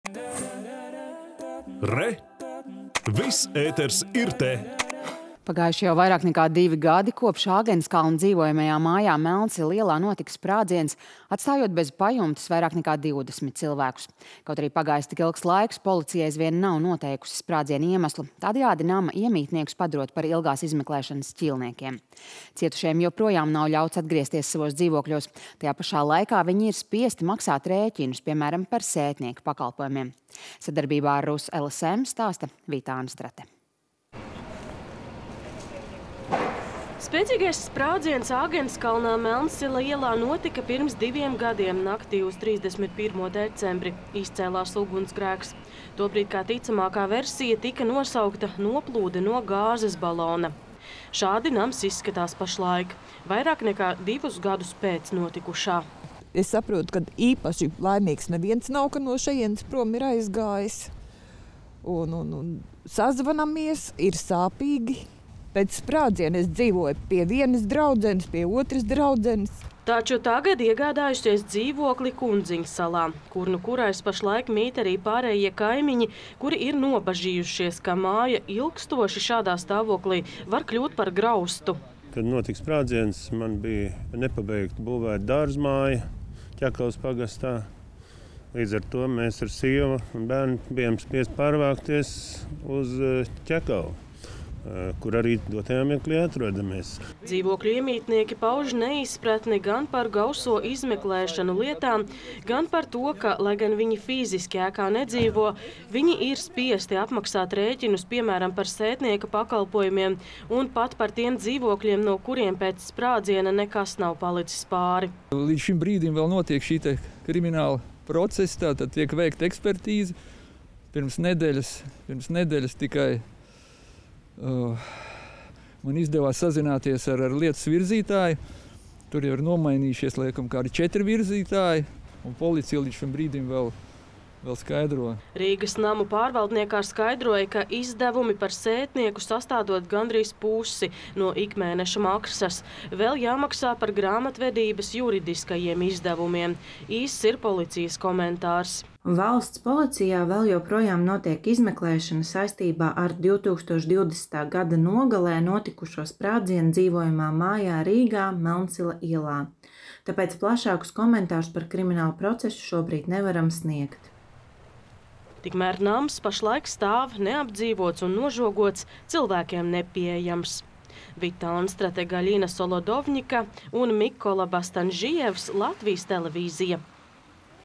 Sprādziens Melnsila ielā – cilvēki divus gadus netiek mājās
Pagājuši jau vairāk nekā divi gadi, kopš Āgenskalna dzīvojamā mājā Melnsila ielā notika sprādziens, atstājot bez pajumtes vairāk nekā 20 cilvēkus. Kaut arī pagājis tik ilgs laiks, policija aizvien nav noteikusi sprādziena iemeslu, tādējādi nama iemītniekus padarot par ilgās izmeklēšanas ķīlniekiem. Cietušajiem joprojām nav ļauts atgriezties savos dzīvokļos, tajā pašā laikā viņi ir spiesti maksāt rēķinus, piemēram, par sētnieka pakalpojumiem. Sadarbībā ar “Rus.Lsm” stāsta Vita Anstrate.